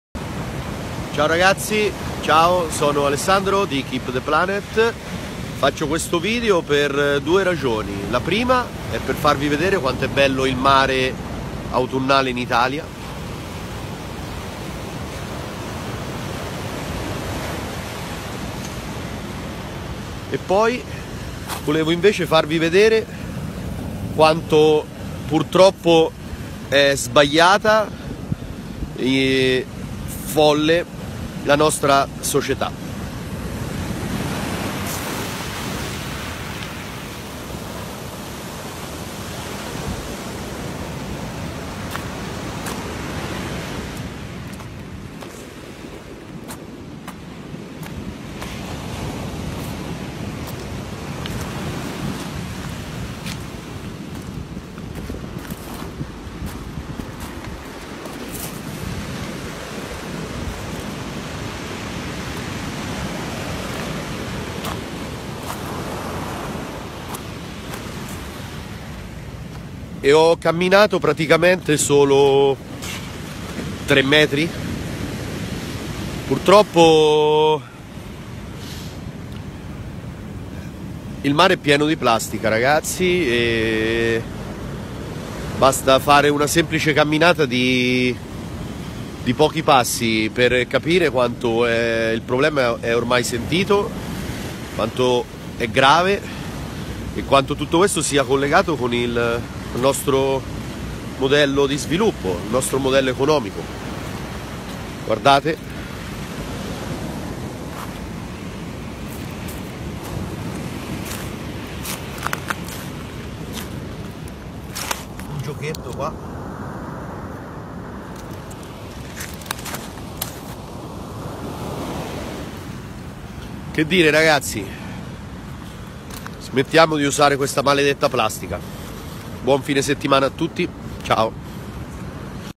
Plastica in Adriatico, smettiamo di usarla
Un breve video su quello che il mare di rida indietro,